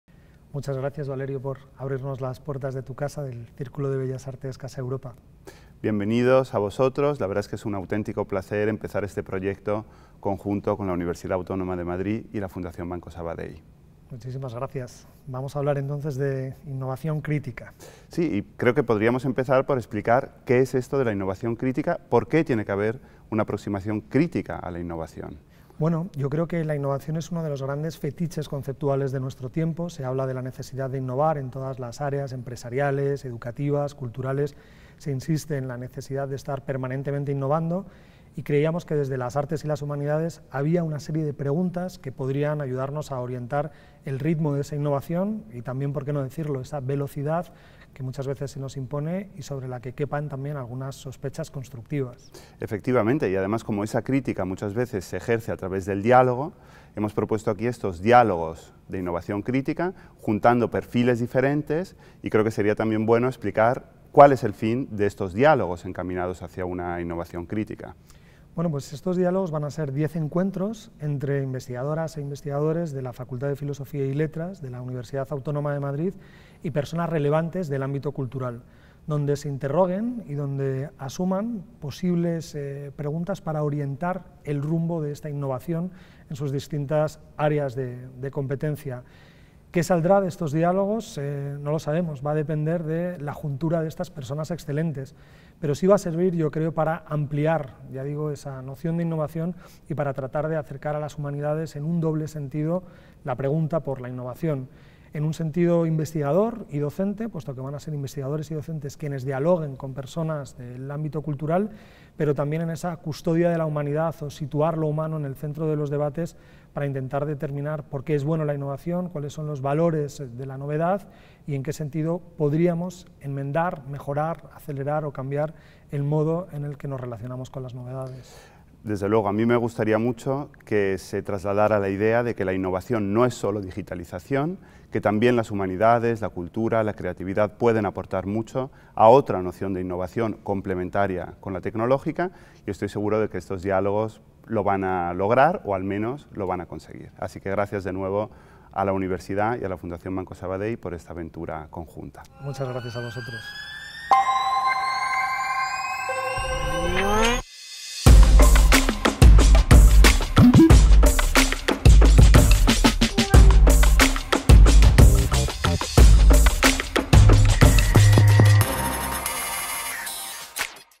Diálogos para una innovación crítica [teaser]
Valerio Rocco, director del Círculo de Bellas Artes, y Diego Garrocho, vicedecano de Investigación de la Facultad de Filosofía y Letras de la Universidad Autónoma de Madrid, nos cuentan el porqué de estos Diálogos para una innovación crítica CulturaInnova, que comienzan este jueves 24 de junio de 2021.

¿Qué impacto social tienen o pueden tener las investigaciones en el campo humanístico/artístico? ¿Y qué posible retorno económico presentan? ¿Cómo se puede evaluar la calidad de la innovación en artes y humanidades? ¿Qué casos interesantes hay de transferencia en nuestros ámbitos? ¿Qué tiene de provechoso, pero qué también puede entrañar de terrible, el aprecio por la estricta novedad que se desprende en la promoción de la innovación?

Diálogos para una innovación crítica es un proyecto que aspira a ejercer un diagnóstico crítico y transdisciplinar de la idea de innovación en el ámbito artístico y humanístico. Para este fin el Círculo de Bellas Artes (CBA) ha planteado diez diálogos en los que investigadores de la Universidad Autónoma de Madrid (UAM) reflexionan y conversan acerca del concepto de innovación con diez personalidades relevantes de diferentes campos del mundo de la cultura.